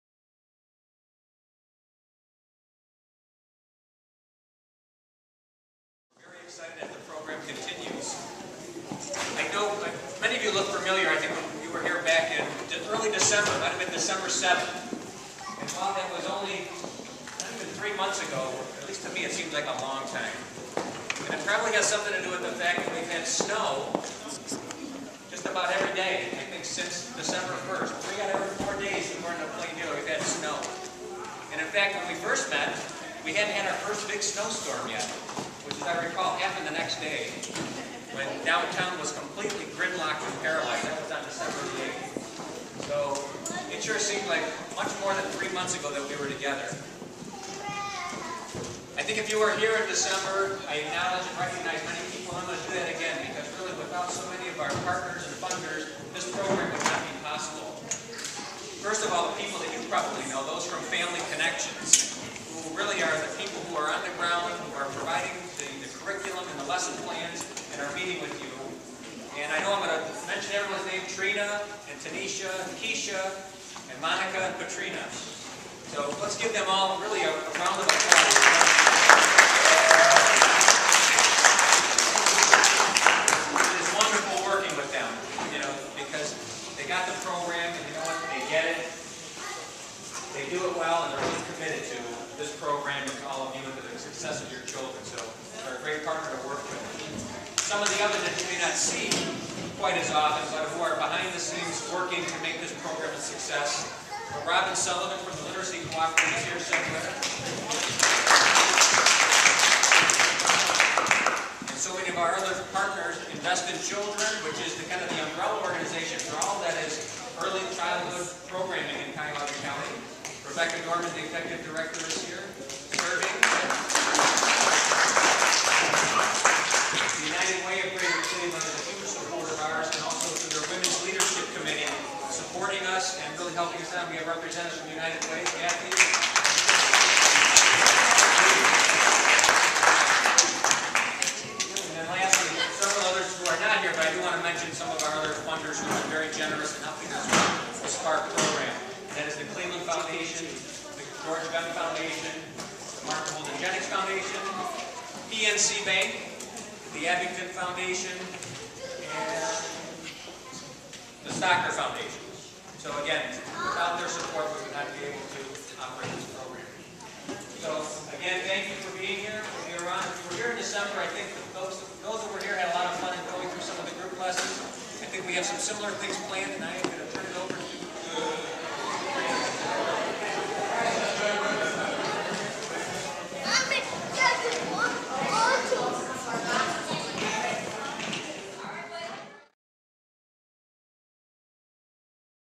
Literacy Cooperative Exec Director addresses crowd at SPARK group lesson
At the opening of the March 1, 2011 SPARK Group Lesson, Literacy Cooperative Executive Director Bob Paponetti took a moment to thank parent partners, sponsors and participants and to recap the year's progress prior to launching into the lesson.